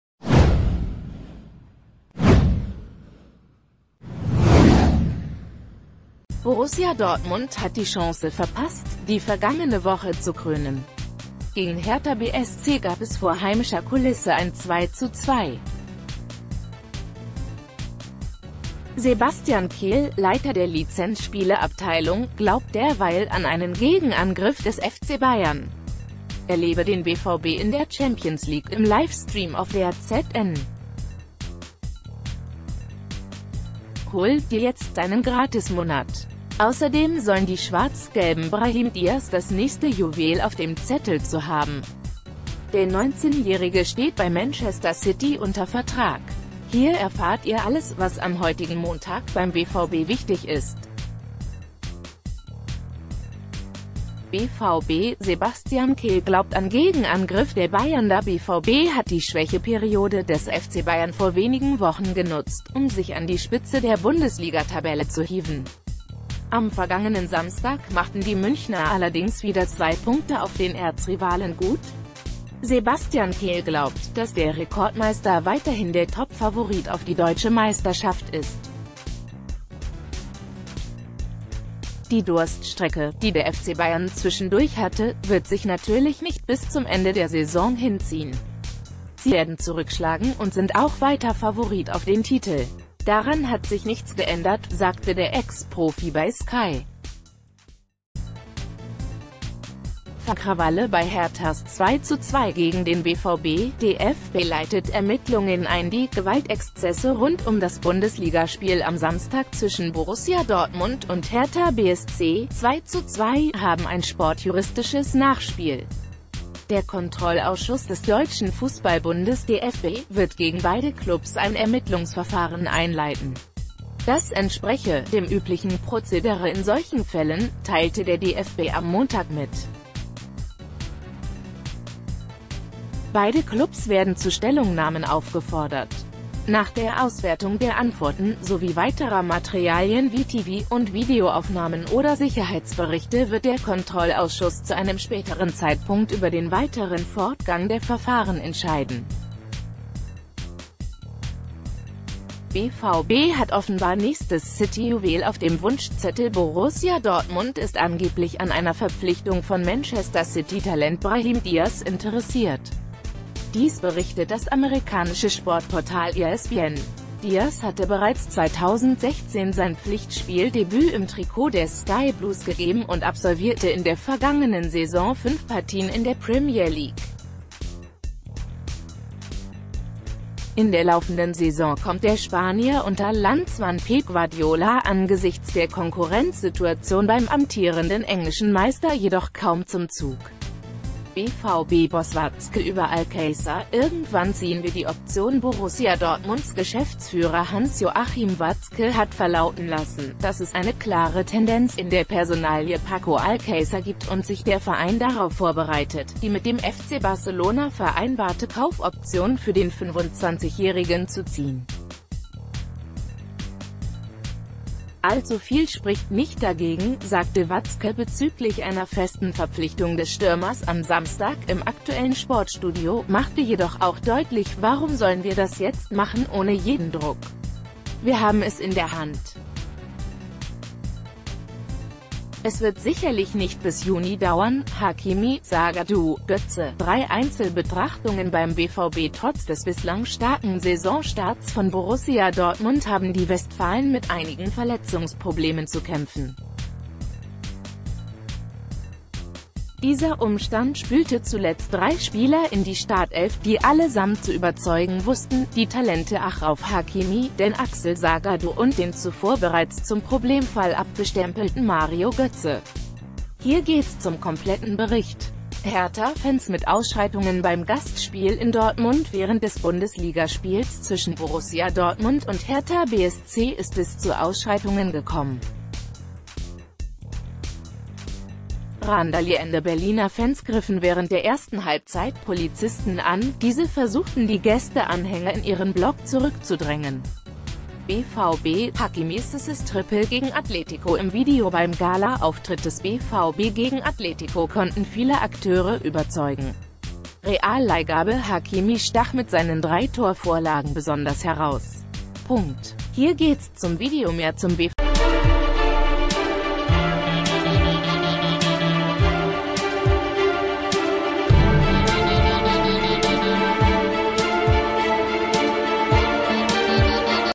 BVB: News und Transfer-Gerüchte zu Borussia Dortmund - Sebastian Kehl glaubt an Gegenangriff des ...
BVB: News und Transfer-Gerüchte zu Borussia Dortmund - Sebastian Kehl glaubt an Gegenangriff des FC Bayern |

Borussia Dortmund hat die Chance verpasst, die vergangene Woche zu krönen. Gegen Hertha BSC gab es vor heimischer Kulisse ein 2:2. Sebastian Kehl, Leiter der Lizenzspielerabteilung, glaubt derweil an einen Gegenangriff des FC Bayern. Erlebe den BVB in der Champions League im LIVE-STREAM auf DAZN. Hol' Dir jetzt Deinen Gratismonat! Außerdem sollen die Schwarz-Gelben Brahim Diaz das nächste Juwel auf dem Zettel zu haben. Der 19-Jährige steht bei Manchester City unter Vertrag. Hier erfahrt Ihr alle...